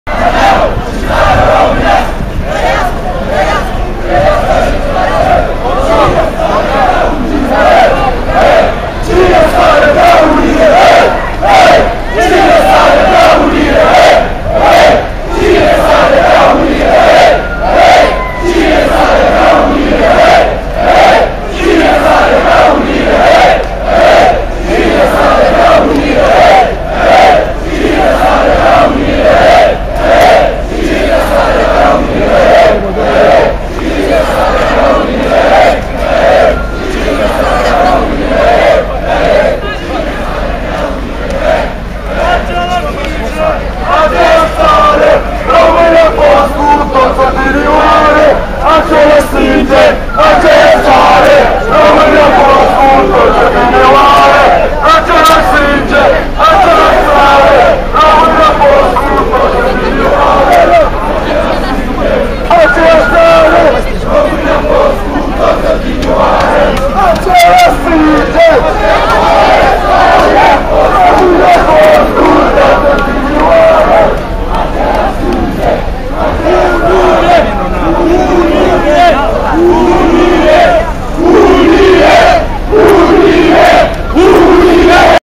Curaj.TV - Scandări unioniste la marșul de comemorare a raptului Basarabiei
Scandări unioniste la marșul de comemorare a 200 ani de la raptul Basarabiei